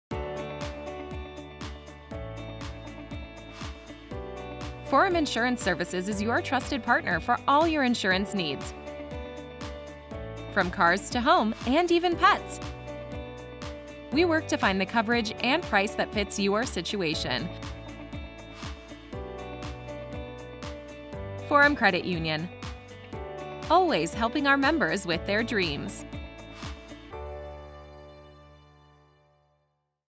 FORUM Insurance Services (30sec OTT)
FORUM Insurance Services is your trusted partner for all your insurance needs, from cars to home, and even pets. We work to find the coverage and price that fits your situation. FORUM Credit Union, always helping our members with their dreams.